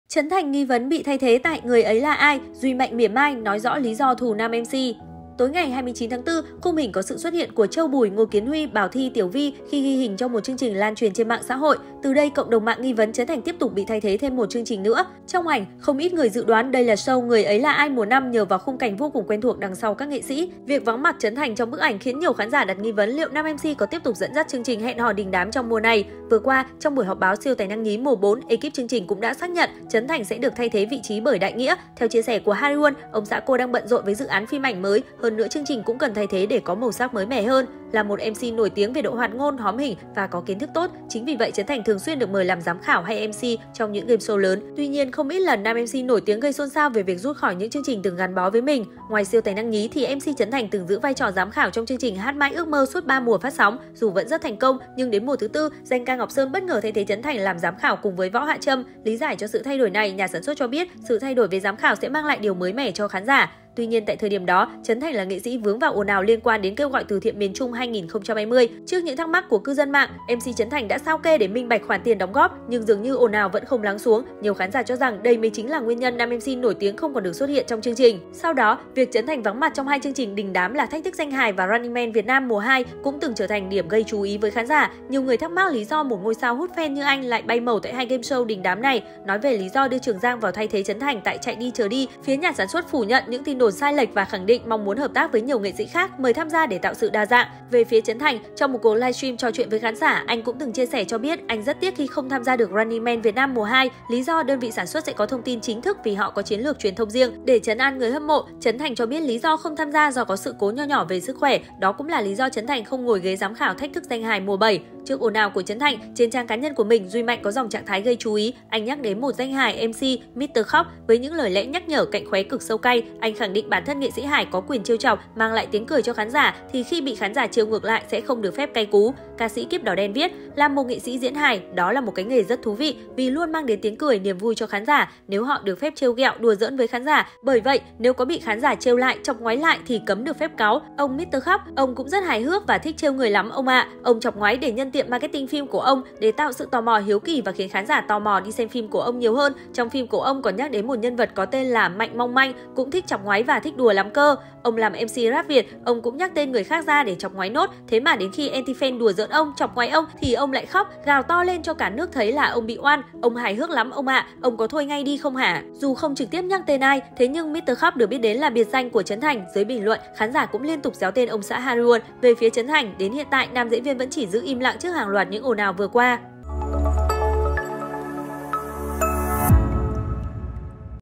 Trấn Thành nghi vấn bị "thay thế" tại Người ấy là ai, Duy Mạnh mỉa mai, nói rõ lý do "thù" nam MC?
Tối ngày 29/4, khung hình có sự xuất hiện của Châu Bùi - Ngô Kiến Huy - Bảo Thy - Tiểu Vy khi ghi hình cho một chương trình lan truyền trên MXH. Từ đây CĐM nghi vấn Trấn Thành tiếp tục bị “thay thế” thêm 1 chương trình nữa.
Trong ảnh, Không ít người dự đoán đây là show Người ấy là ai mùa 5 nhờ vào khung cảnh vô cùng quen thuộc đằng sau các nghệ sĩ. Việc vắng mặt Trấn Thành trong bức ảnh khiến nhiều khán giả đặt nghi vấn liệu nam MC có tiếp tục dẫn dắt chương trình hẹn hò đình đám trong mùa này.